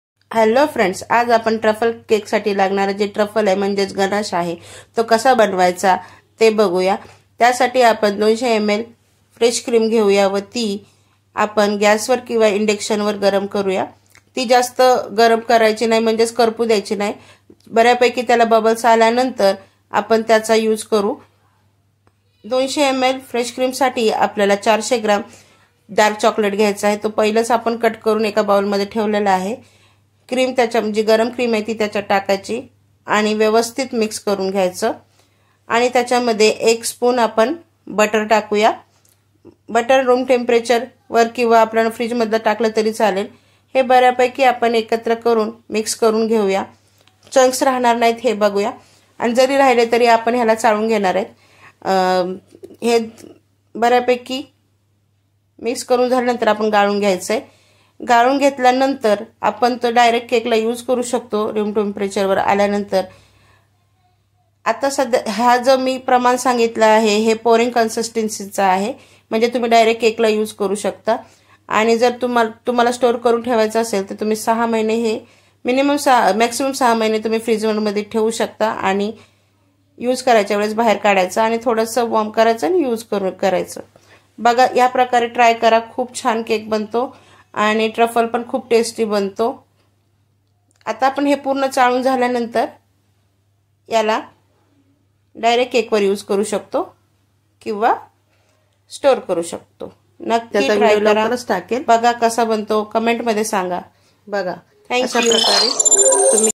Choclate ganache|Truffle cake| चॉकलेट केक साठी लागणारा गणाश कसा बनवायचा आणि स्टोअर कसा करायचा
Choclate ganache|Truffle cake| youtubevideo cake truffle trufflecake homemade chocolatecake Choclate ganache|Truffle cake| चॉकलेट केक साठी लागणारा गणाश कसा बनवायचा आणि स्टोअर कसा करायचा

ingredients

200 ml         fresh cream

400 gram    dark chocolate

1Tbsp.          butter

Ganash store in freezer maximum 6 month.